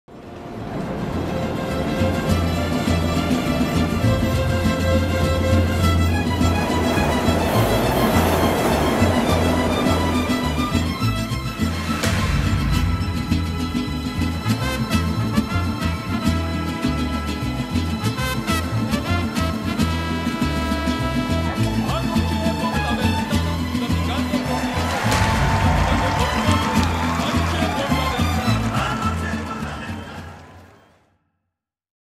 LaMusicaRompeFronteras
Esto es lo que ocurre cuando México llega con uno de sus símbolos culturales más importantes a otro país. Orgullo, tradición, música y Mariachi para el mundo.